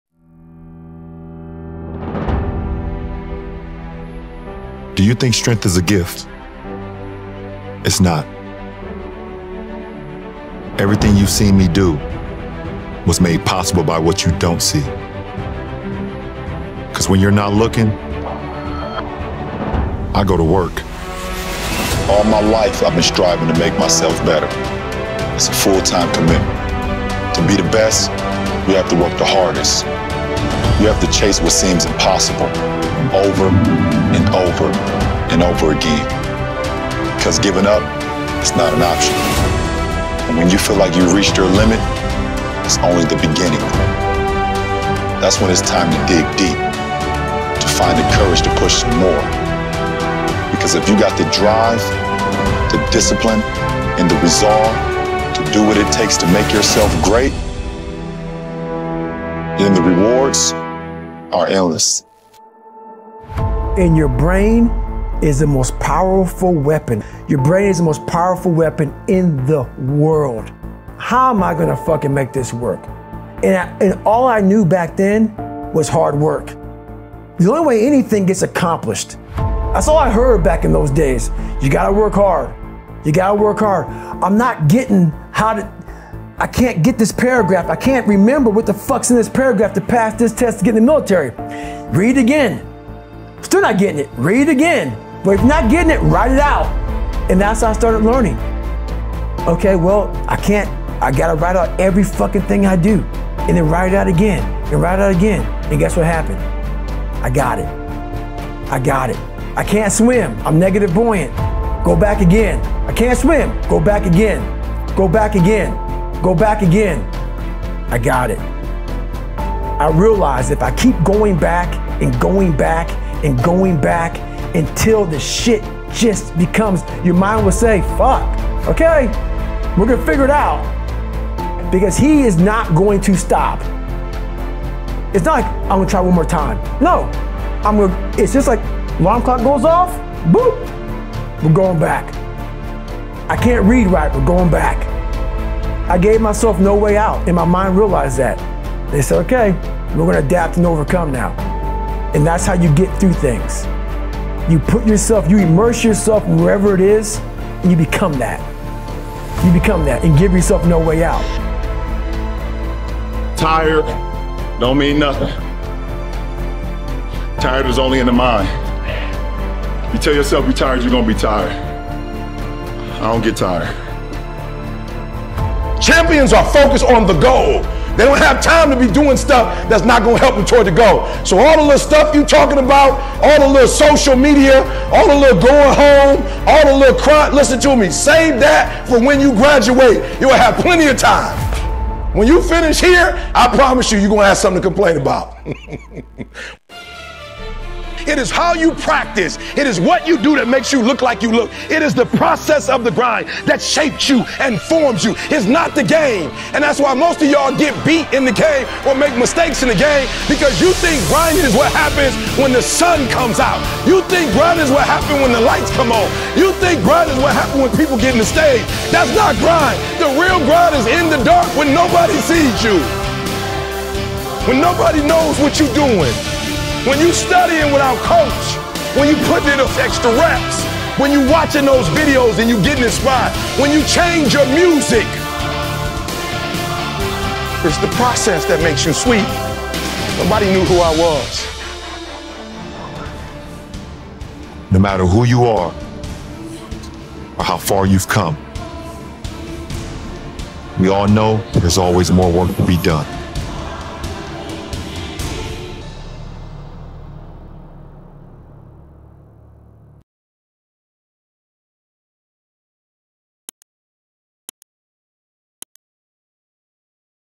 FOCUS ON THE GOAL - Motivational Video | LeBron James | David Goggins | Eric Thomas
''You tell yourself you're tired, you gonna be tired. I don't get tired.''

Focus On The Goal Motivational Video
Edited by @wedontgettired

Spoken by;

1. LeBron James

2. David Goggins

3. Eric Thomas

Stay in lionmode